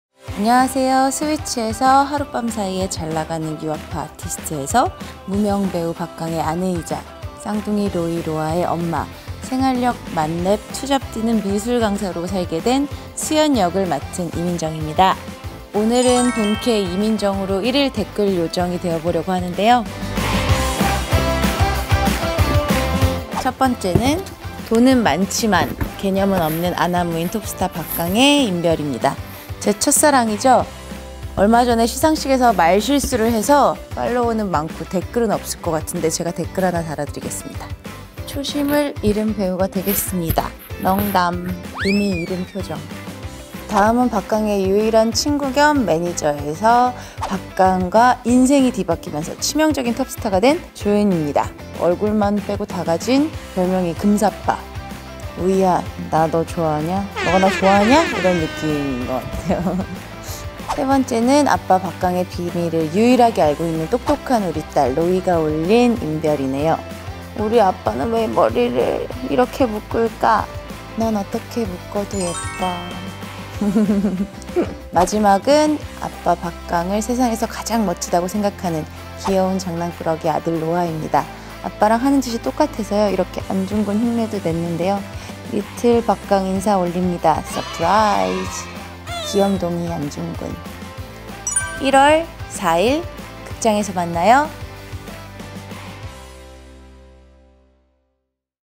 [스위치] 홍보 요정 이민정 등판
대한민국 대표 연쇄댓글마(?) 등장!
스위치 팸에 댓글을 남긴다면?